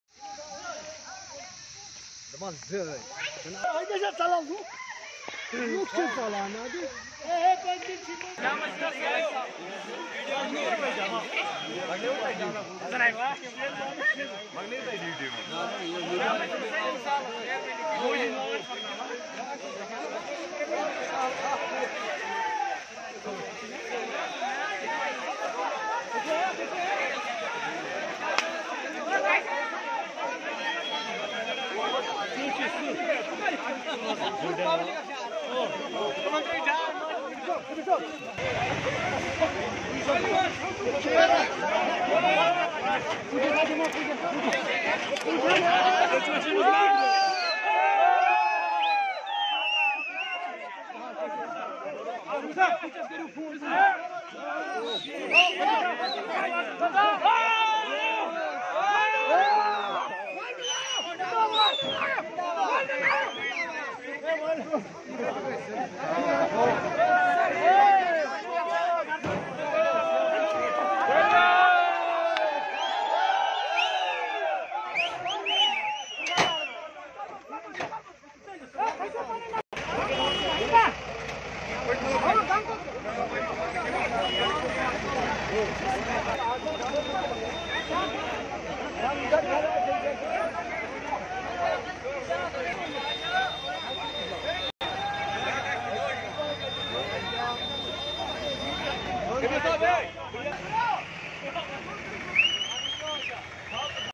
A wild bear visited a village in central Kashmir. Here is what happened next | The Dispatch
Bear caught  by wildlife department with the help of locals in Ganderbal.

A bear was wandering in Babagund area of Ganderbal district. People gathered in hundreds and attacked the bear with sticks. Wildlife authorities also used their nets and injections. The bear was caught successfully. People are in relief now because the bear had made huge loss to fruits. People also thanked the Wildlife authorities.